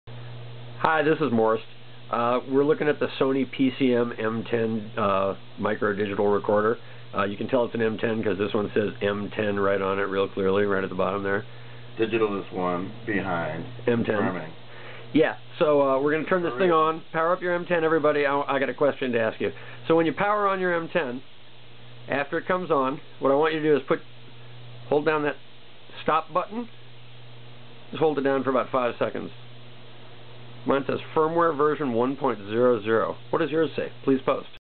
Sony PCM-M10 Firmware check - Version 1.00 or not?
Do you have Firmware Version 1.00? Hold the STOP key to find out.
To find out the firmware version of your Sony PCM-M10 solid state audio recorder, power the machine up, then press and hold the STOP button. After about 5 seconds,, the screen will display the current firmware version. My PCM-M10/BLC is a black color model, from USA. It reports Firmware Version 1.00. If yours differs, please post here.